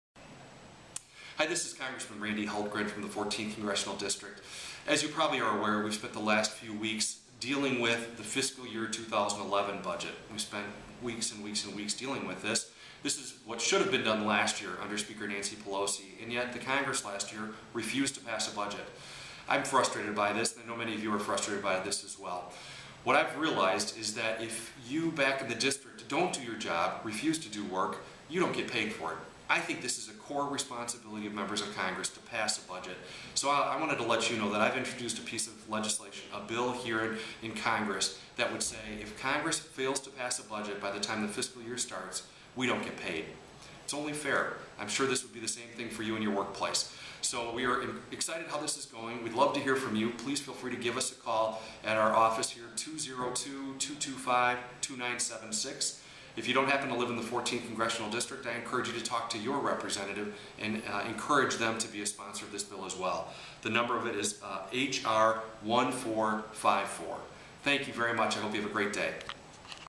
Randy Hultgren Discusses The Congressional Pay Accountability Act
Congressman Randy Hultgren discusses the Congressional Pay Accountability Act, his legislation which would require Congress to pass a budget in order to get paid.